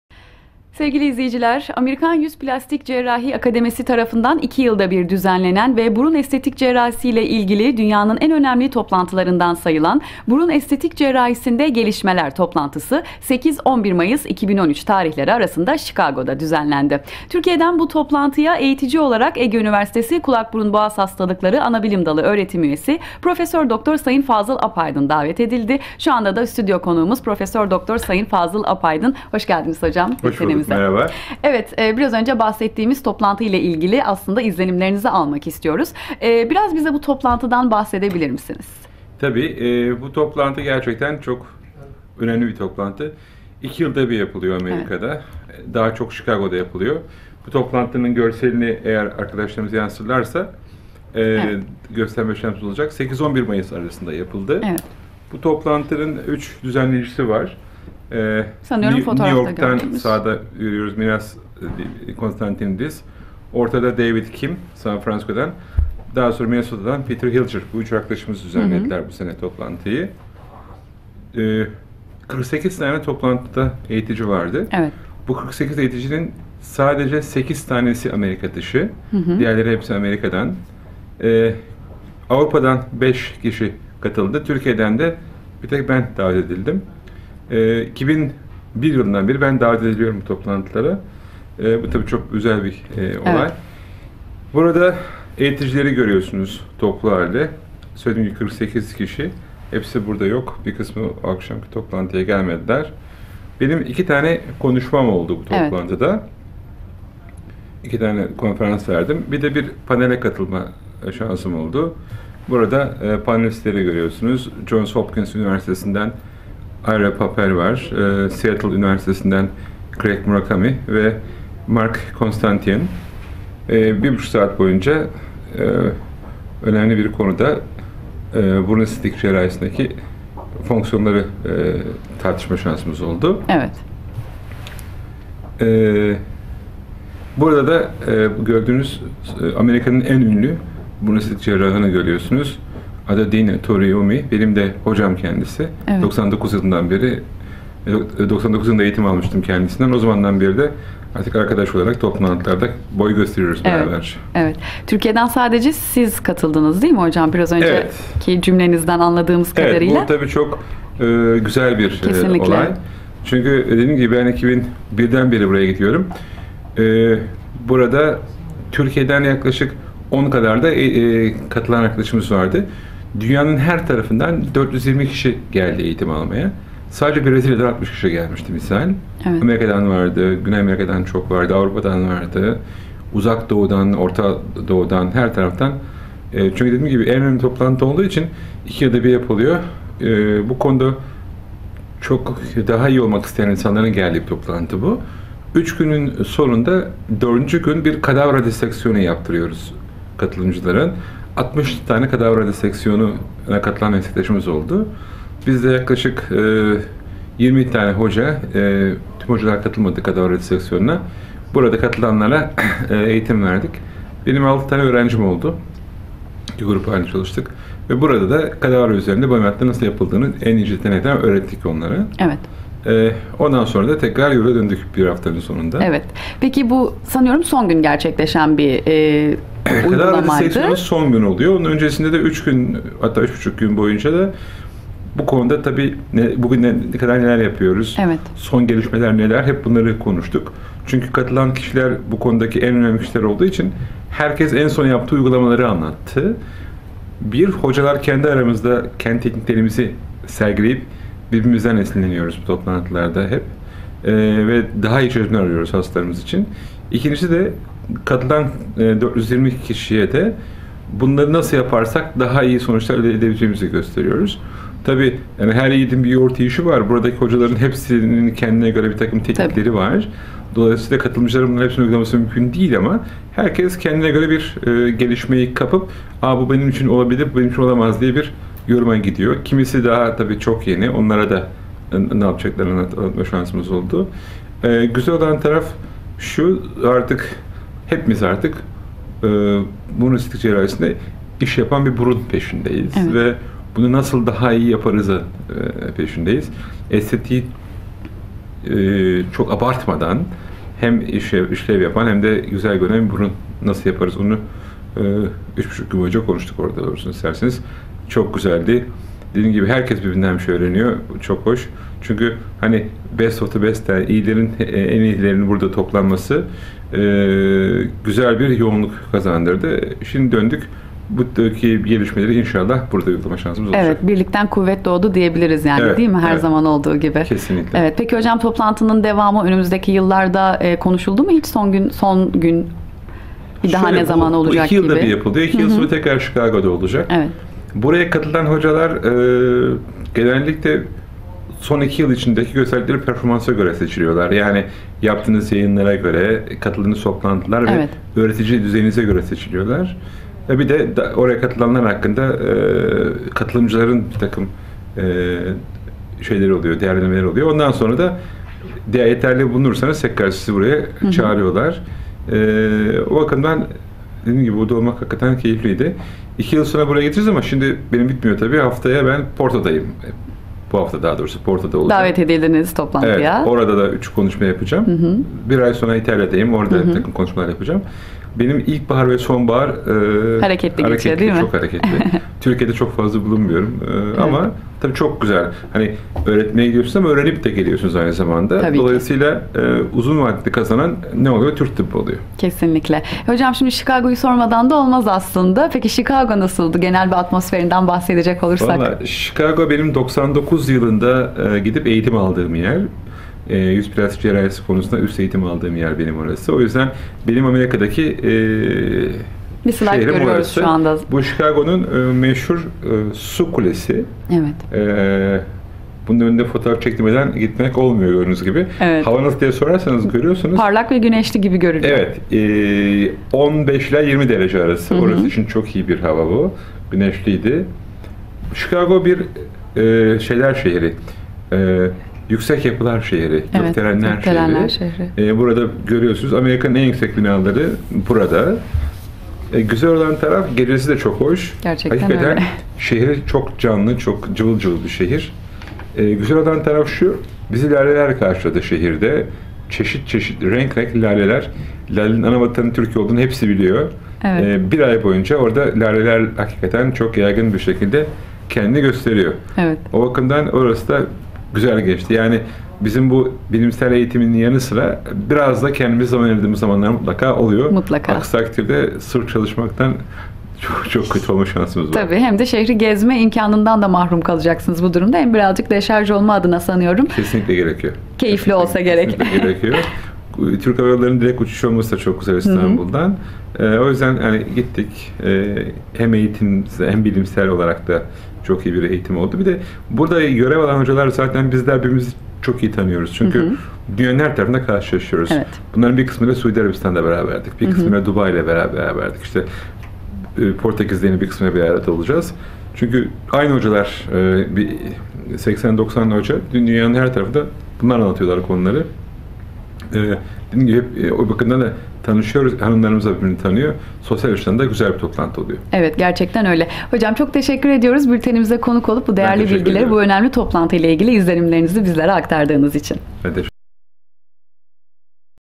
Amerikan Yüz Plastik Cerrahi Akademisi
Amerikan Yüz Plastik Cerrahi Akademisi tarafından 2 yılda bir düzenlenen ve burun estetik cerrahisi ile ilgili olarak dünyanın en önemli toplantılarından sayılan "Burun Estetik Cerrahisinde gelişmeler" toplantısına katılan, Tıp Fakültesi Kulak Burun Boğaz Anabilim Dalı Öğretim Üyesi Prof. Dr. Fazıl Apaydın, öğle haber bültenimizde izlenimlerini aktardı.